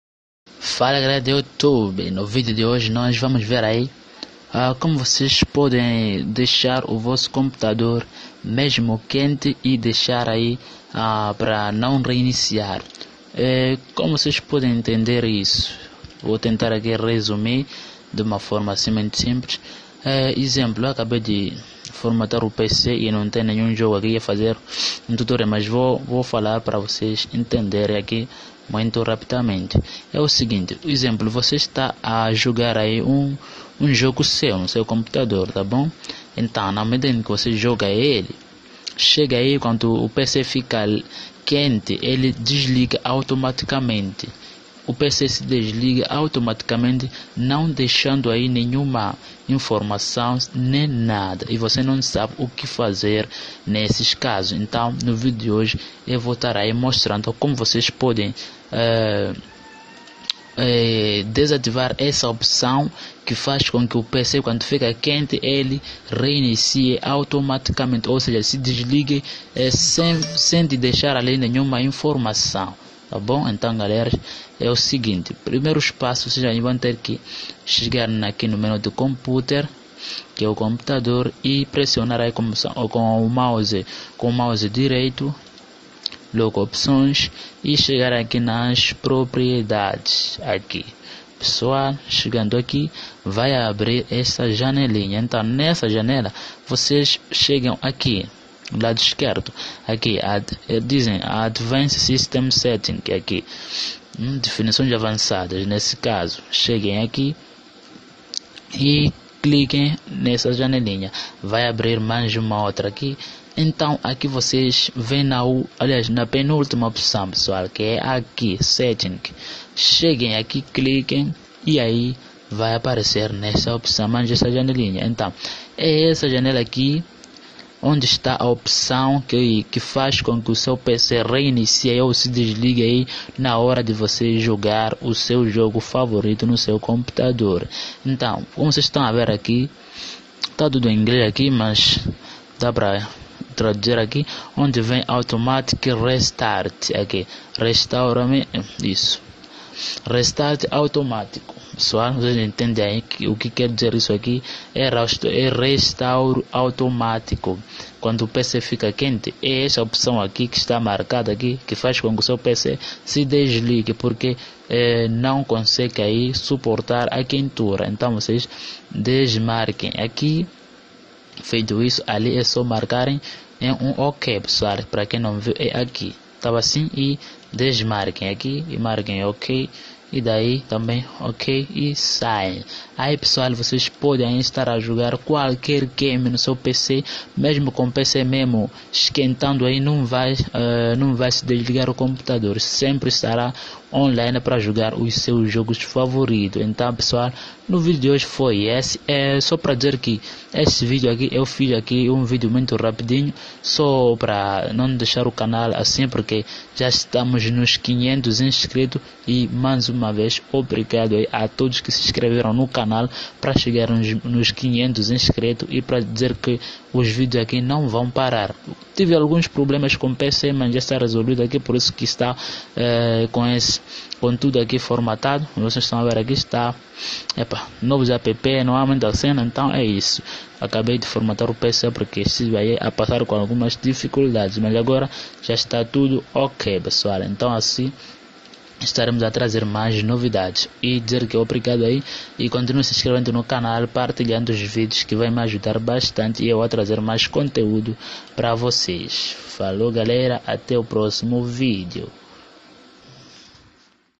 PC REINICIANDO SOZINHO QUANDO O JOGO E ABERTO RESOLVA AGORA
Fala galera concerteza si voce clicou nesse video e Porque esta com esse problema ne ?

   Sabe aquela hora que voce decide jogar aquele jogo desejado tipo san andreas  gta vice city aquele jogos pesados e logo que entra e o seu notebook ou desktop logo si desliga sem dar nenhuma informação ?

Entao nesse video vamos resolver junto esse problema ta pessoal
so para dizer que nao vamos precisar de nenhum programa apenas com o seu PC voce vai resolver esse problema e vamos.